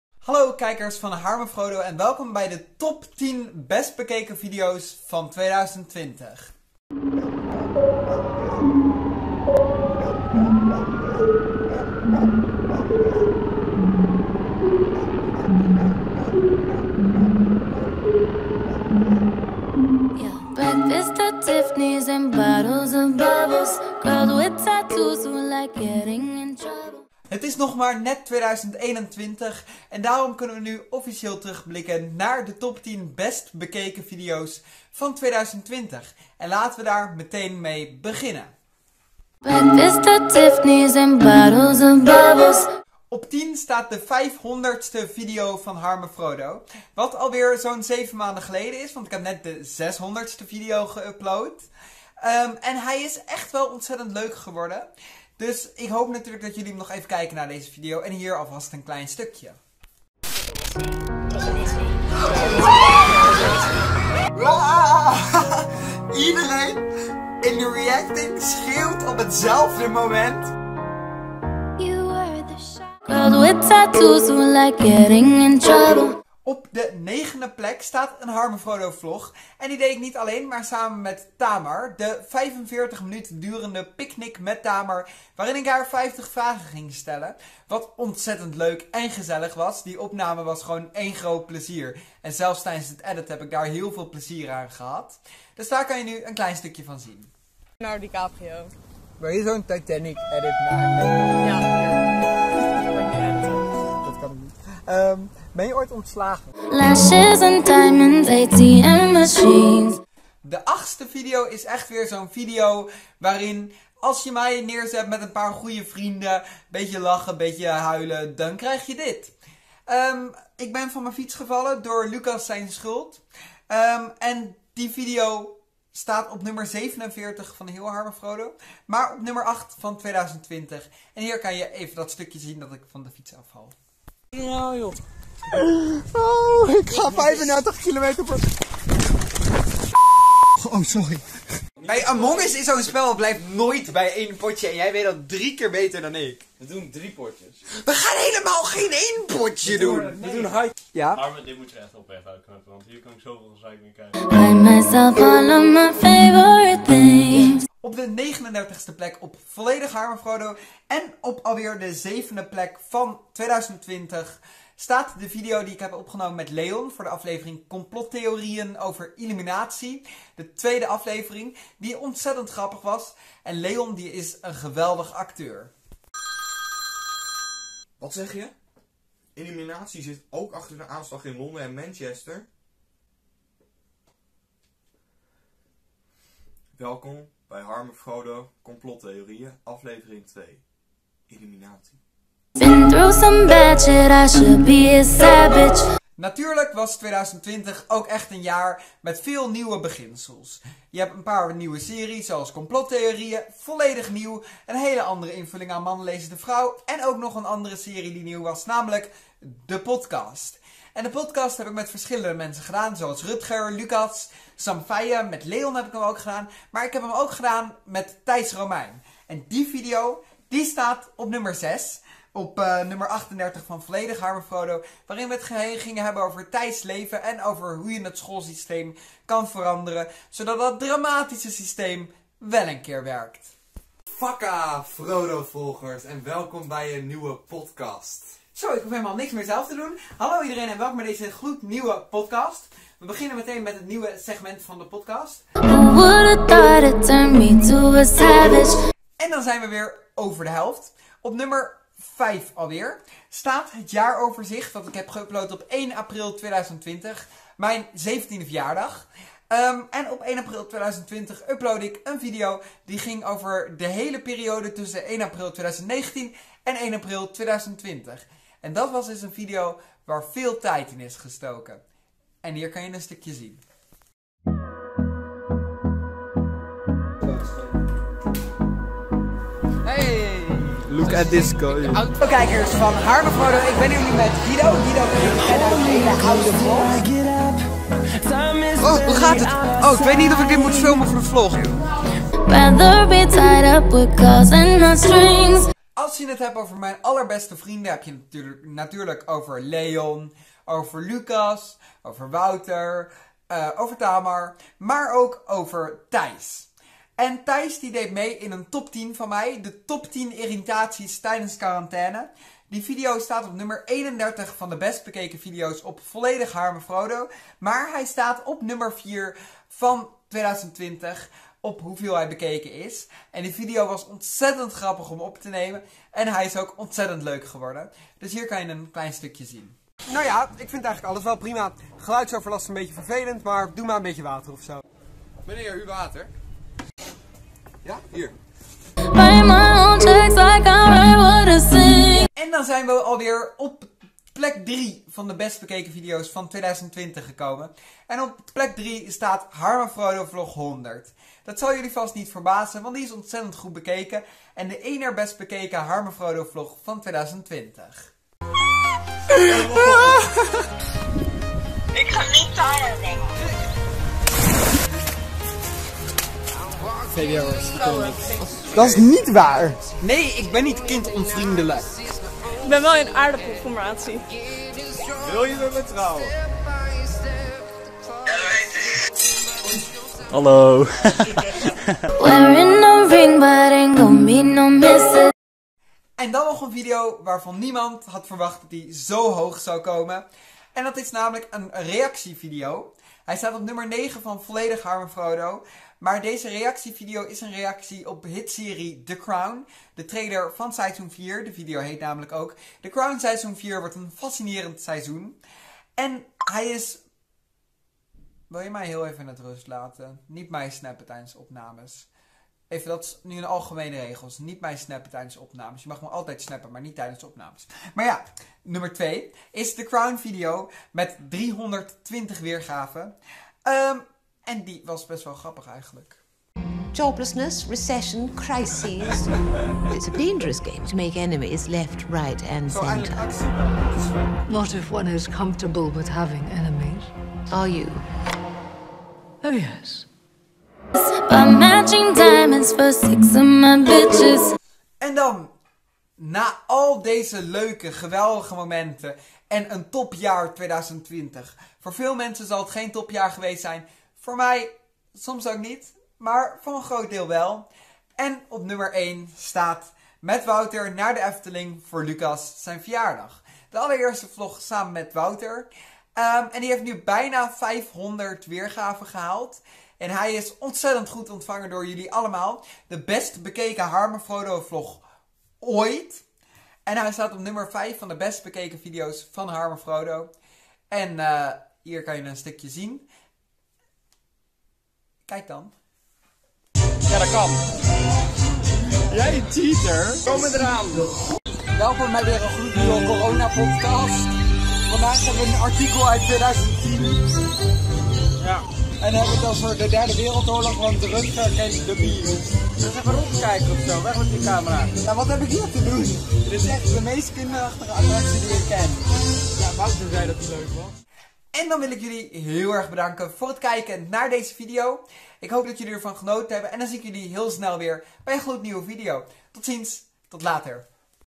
TIEN BEST BEKEKEN VIDEOS VAN 2020!!!
✔ Hey en leuk dat je kijkt naar weer een gloednieuwe top tien: De top tien best bekeken video’s van 2020.

✔ Uploads elke maandag (16:00), soms woensdag (16:00) en soms vrijdag (16:00) in HD-kwaliteit (720/ 1080 px)